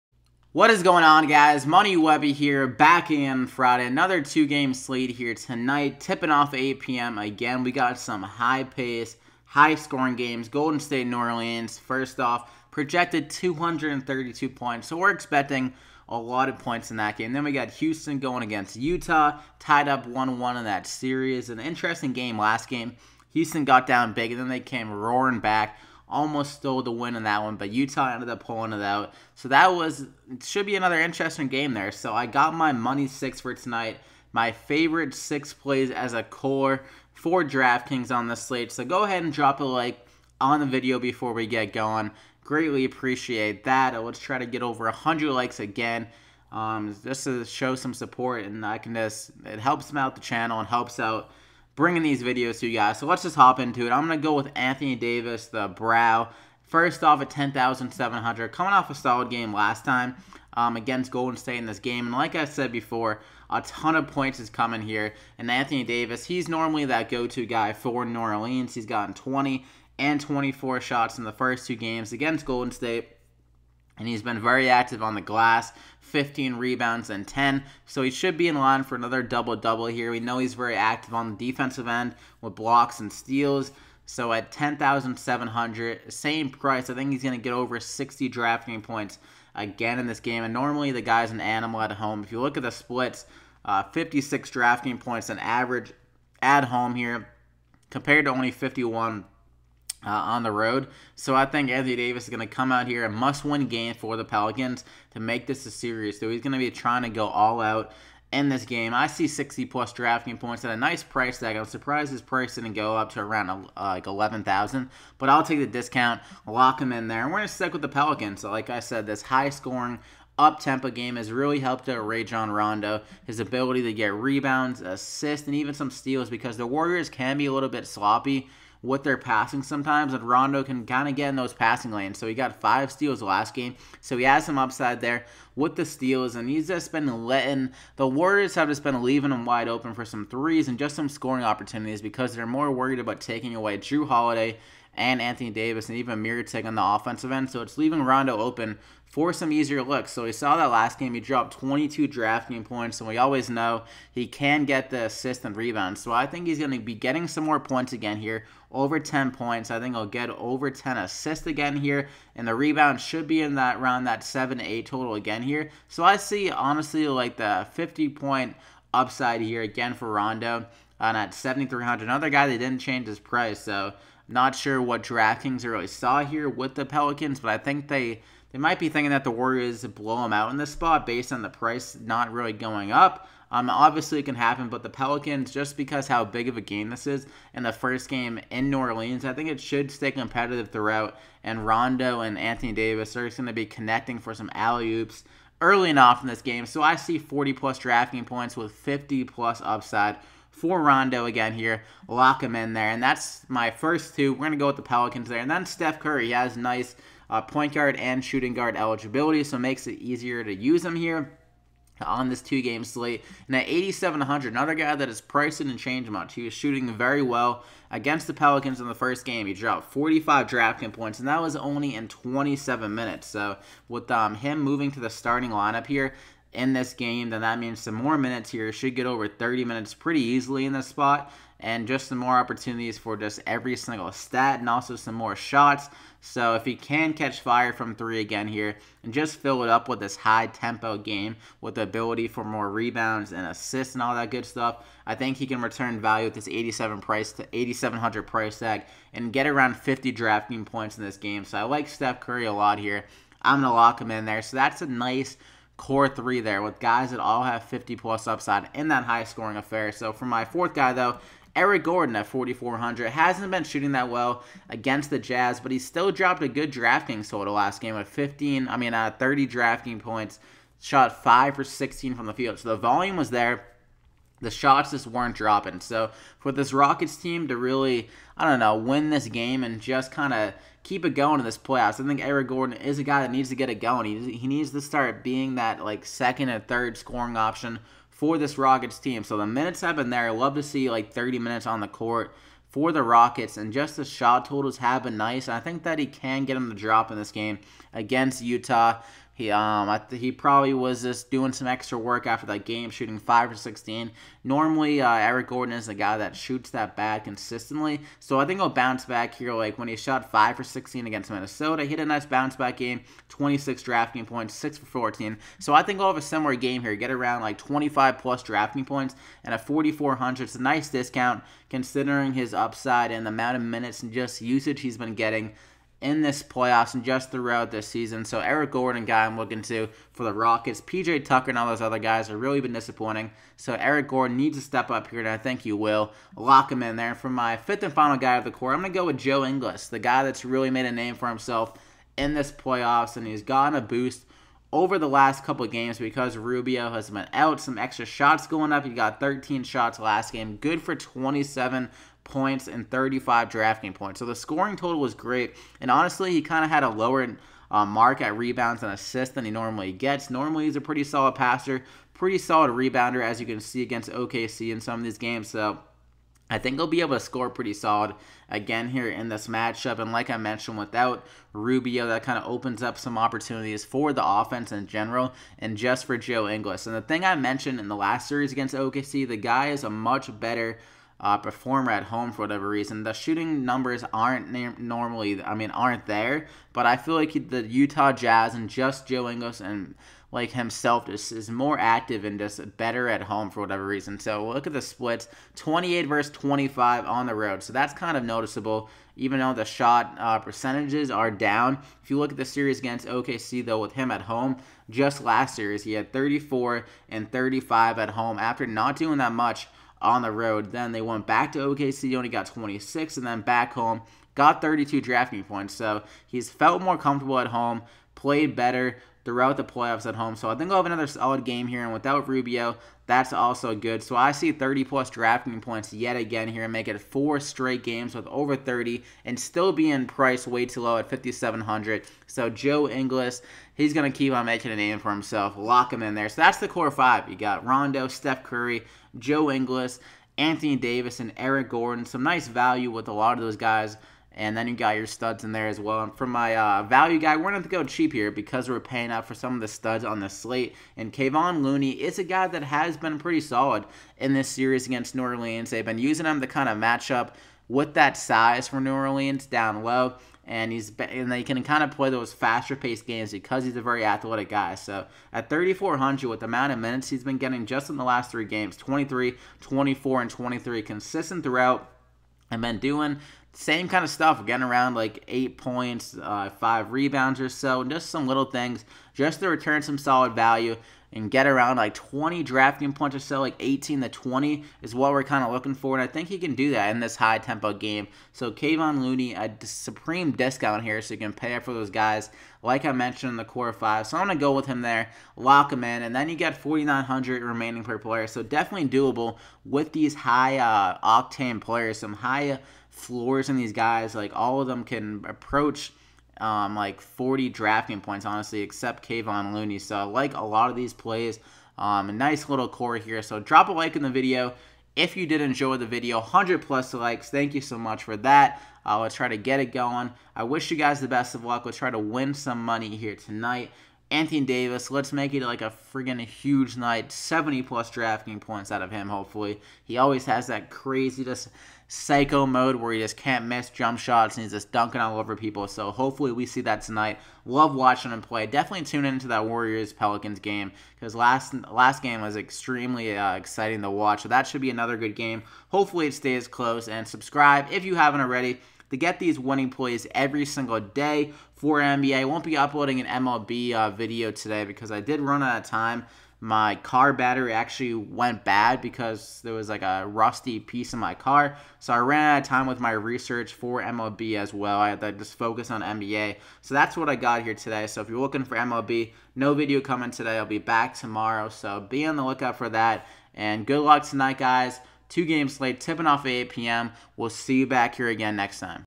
5/4/18 NBA Playoffs DraftKings Picks - Money Six
My Favorite Plays Of The Day With Some Cheap Options, Sleepers, Must Plays, Value Plays for the 5/4/18 NBA Playoffs Slate for DraftKings.